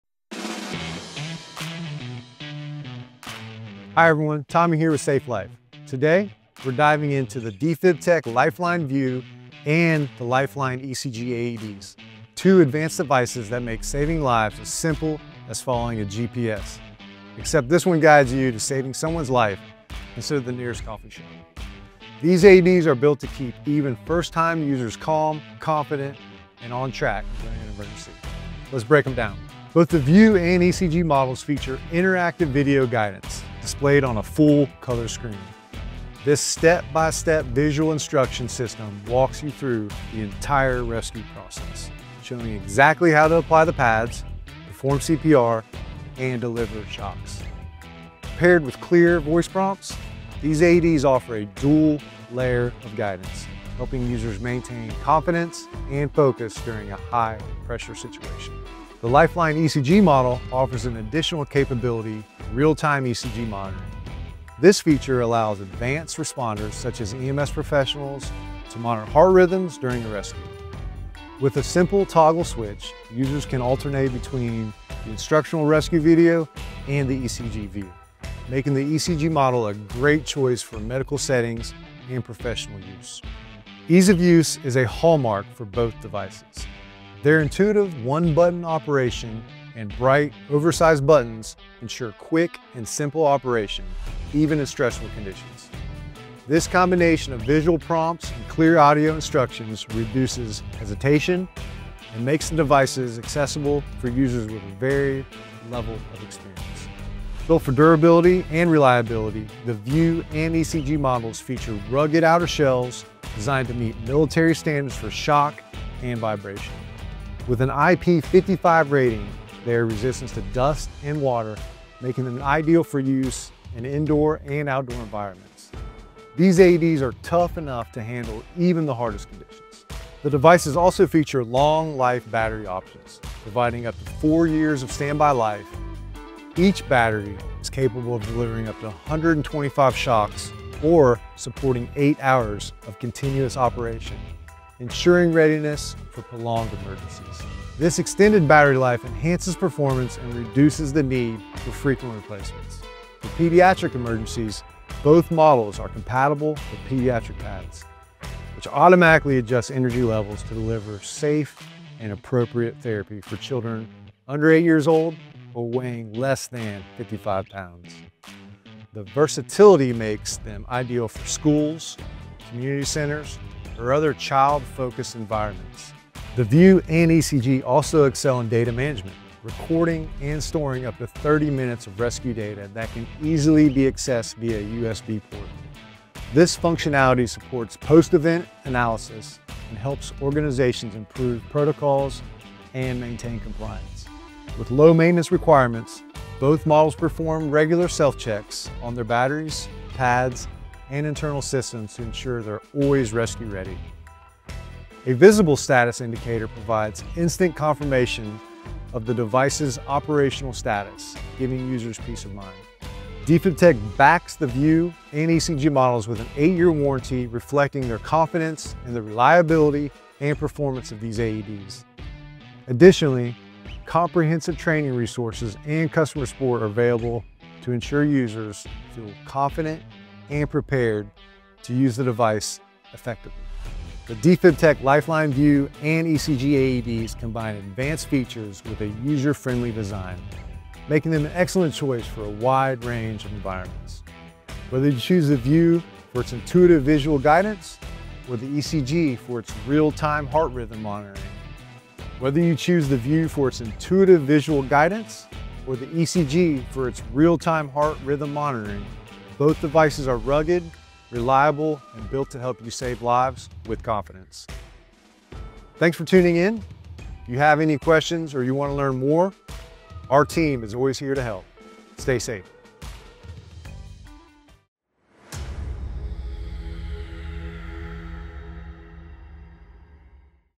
Defibtech Lifeline VIEW AED: Comprehensive Review & User Guide
Want an AED with crystal-clear, step-by-step guidance? The Defibtech Lifeline VIEW AED stands out with its full-color video screen that walks users through every step of CPR and defibrillation. In this video, we explore how this user-friendly AED combines cutting-edge visuals with strong performance and simple maintenance.

Don’t wait until it’s too late. Be prepared to save a life today! Stay safe! 🚨